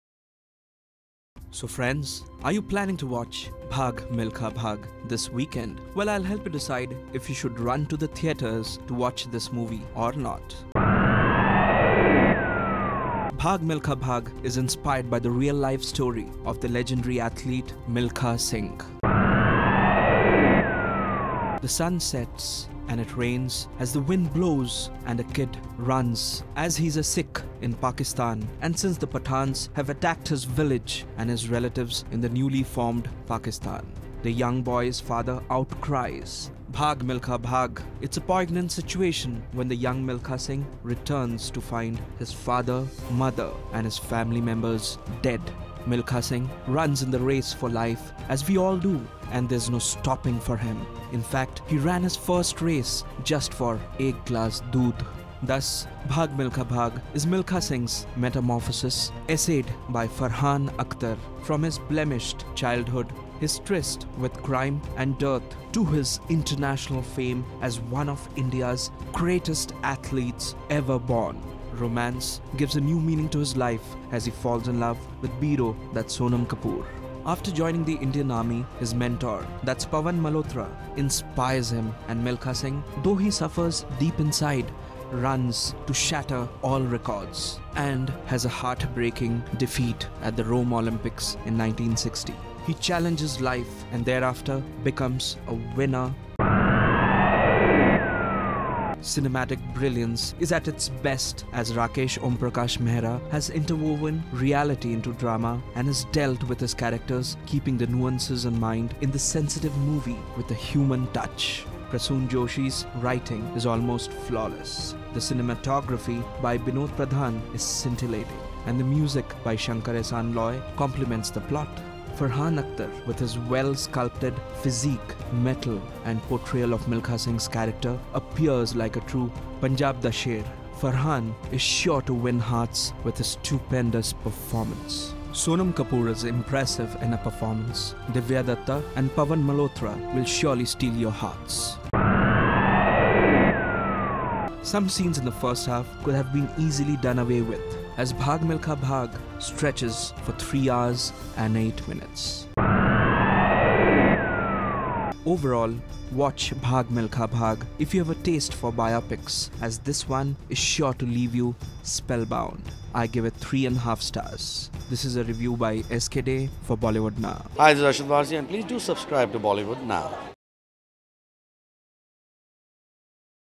Bhaag Milkha Bhaag Review Movie Reviews
Bhaag Milkha Bhaag Review Movie Reviews. Bhaag Milkha Bhaag is a biographical sports movie based on the life of Milkha Singh who's known as The Flying Sikh, Bhaag Milkha Bhaag stars Farhan Akhtar, Sonam Kapoor, Meesha Shafi,Pawan Malhotra,Divya Dutta. Watch the Bhaag Milkha Bhaag Review by S.K. De to find out if you should pause in your fast paced life to watch this one or not.